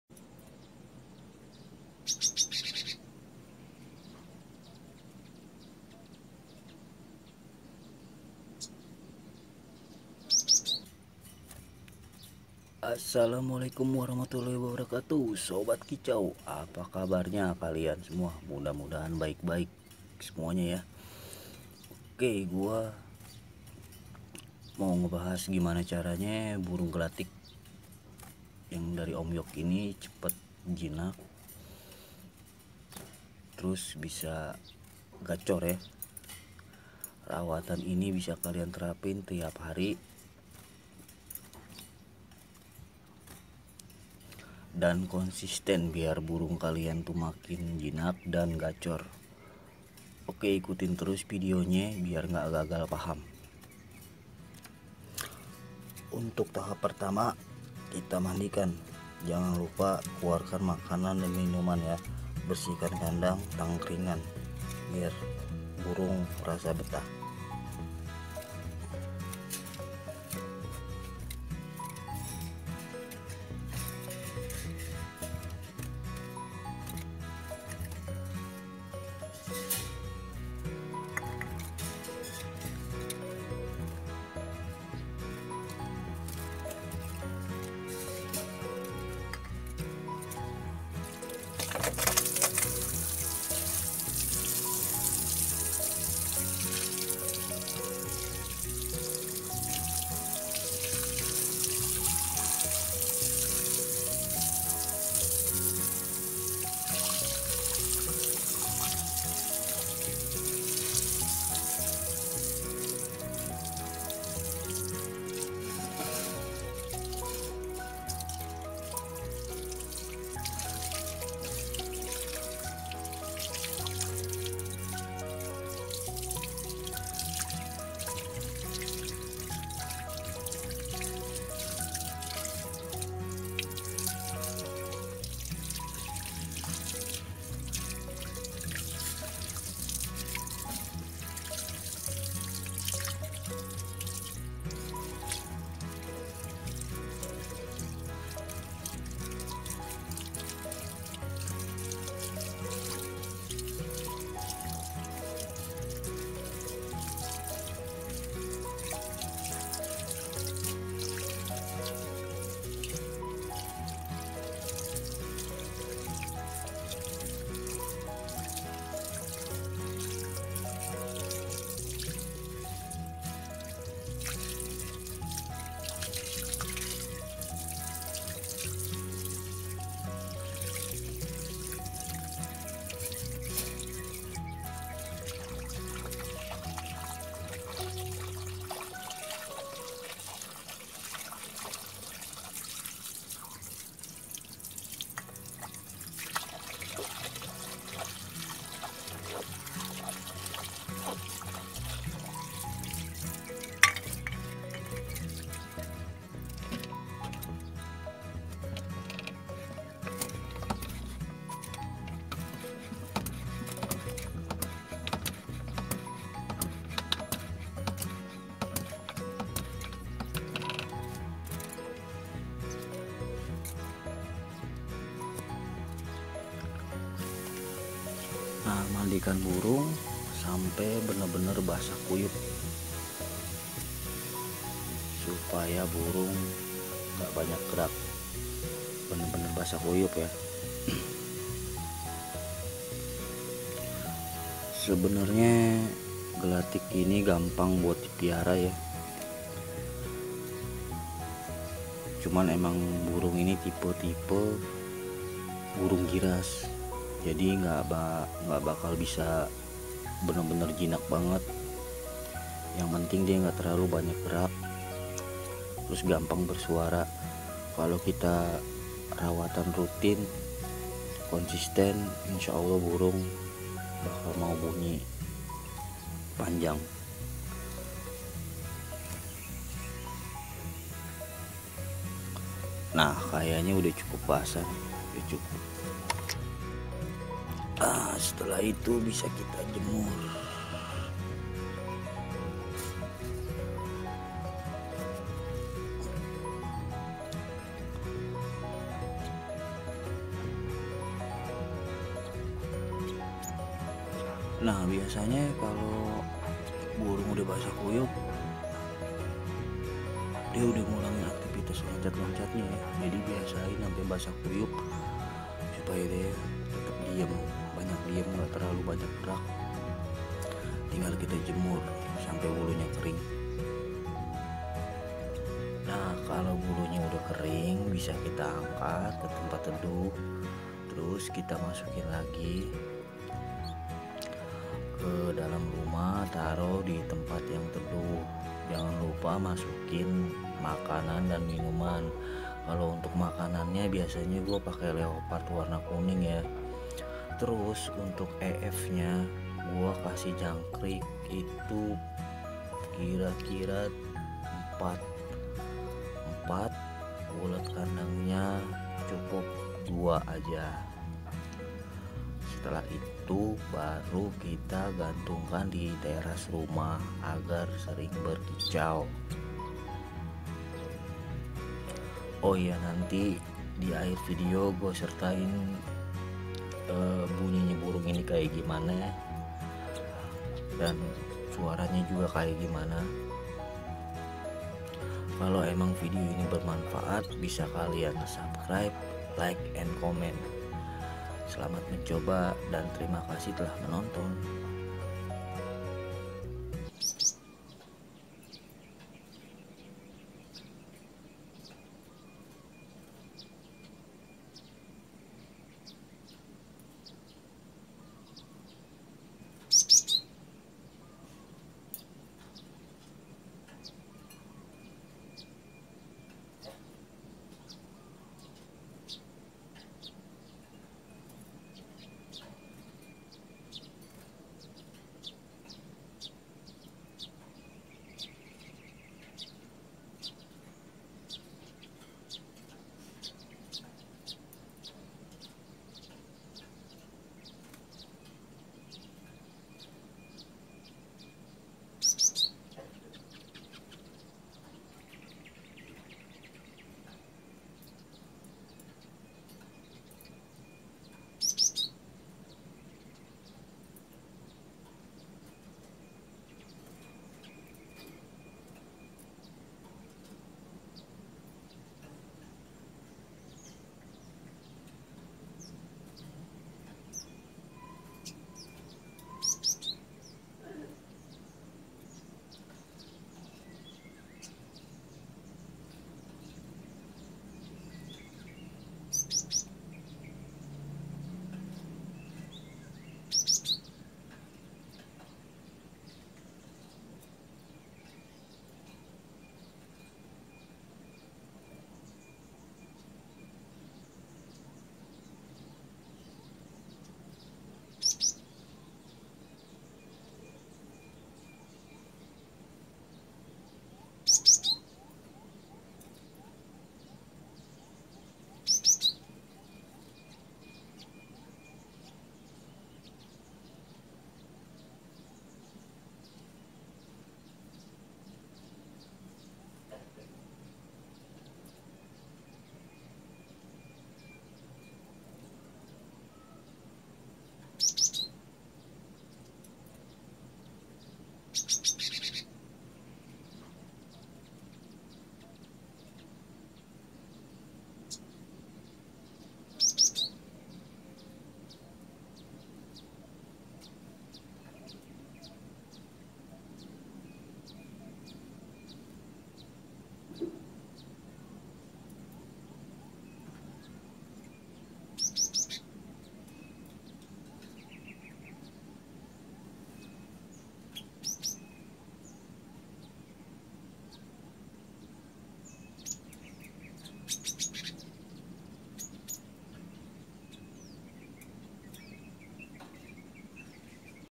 Cara merawat gelatik batu dari ombyokan | pasti jinak & gacor
Cara merawat burung gelatik batu dari ombyok agar cepat bunyi.
dengan rawatan harian yang konsisten burung gelatik batu bisa cepat jinak dan gacor